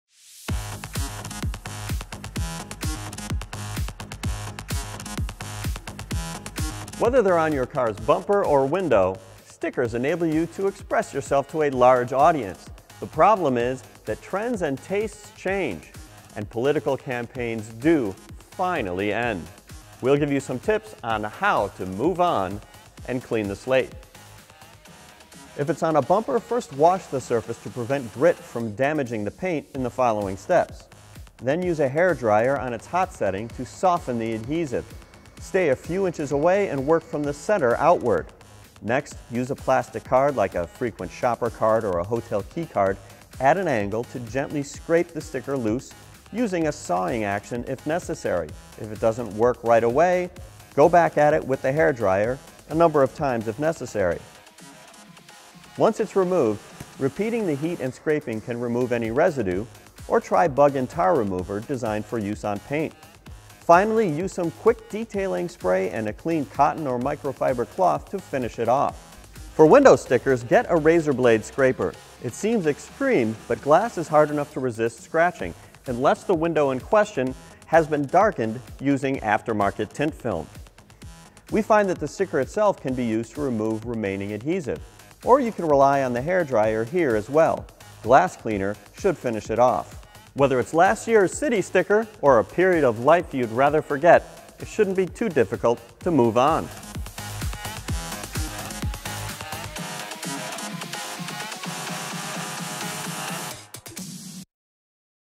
How Do I Remove a Sticker From My Car?
Whether they're on your car's bumper or a window, stickers enable you to express yourself to a large audience — but the problem is that trends and tastes change, and political campaigns end. Cars.com’s Joe Wiesenfelder shows how to clean the slate in the video above.

Chapters:
0:00 Intro
0:25 Remove stickers from a bumper
1:09 Remove stickers from a window
1:21 Final tips

Like what you see? Follow Cars.com!

Visit us for the latest car news, videos and expert reviews